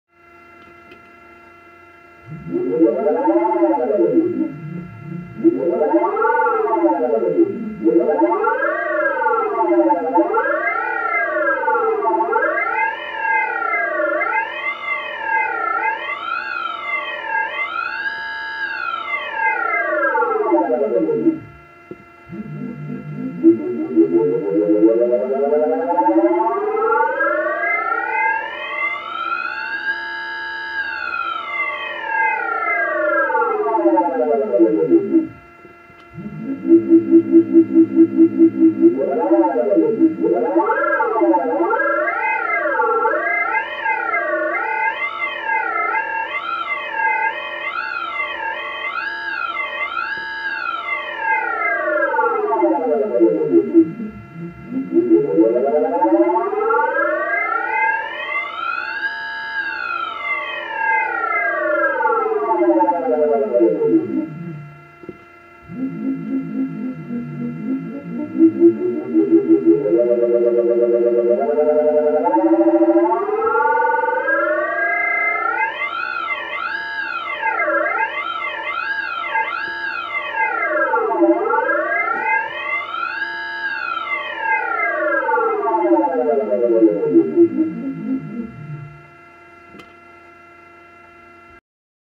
Sound only 自作効果音 とにかく変な音 高音域で猫のうなり声っぽい
偶然できた変な効果音。高音域では猫のうなり声のような音色が
混ざって聞こえる。音の世界はまさに無限大。